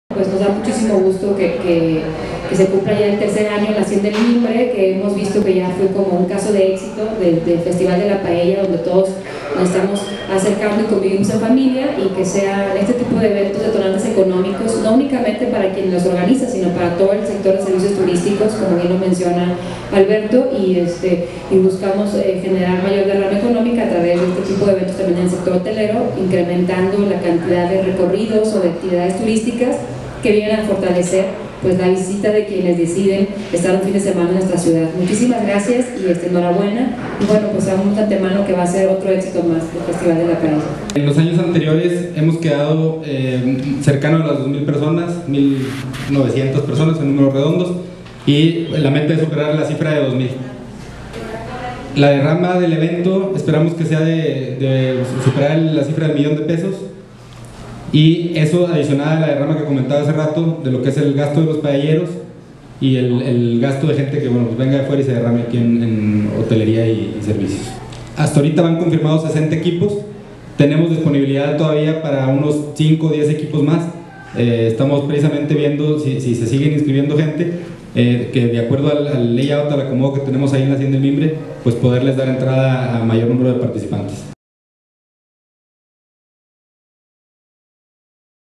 INVITAN A VIII FESTIVAL DE LA PAELLA
El próximo 2 de julio se realizará el octavo Festival de la Paella de Saltillo, en las instalaciones de la Hacienda El Mimbre al sur de la ciudad. Alberto Salinas y Elisa Siller presentaron este evento al que invitaron a las familias del país a asistir. (JOSÉ TORRES)